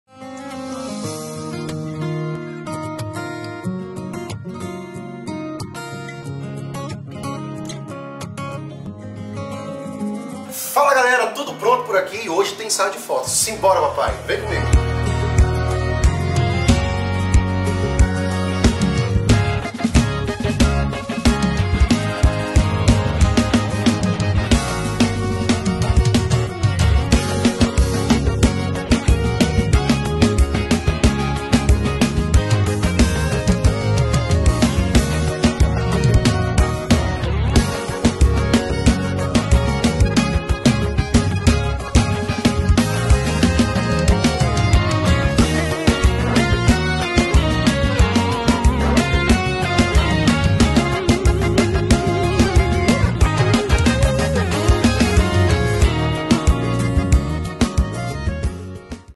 Luccas Fernandes - Making Of - Ensaio Fotográfico
Making of do ensaio fotográfico para o meu novo presskit!

Ficha técnica:
Realização: Universidade do Artista
Direção: Juliano Kalcinha
Fotos: Eduardo Carvalho
Vídeo: Lucas Hoffman
Produção: Vinicius Araujo e Ian Castilho
Mídia: Ian Castilho
Make: Alan
Cabelo/Barba: @ge_barbershop
Violão: @toledomusicalcampinas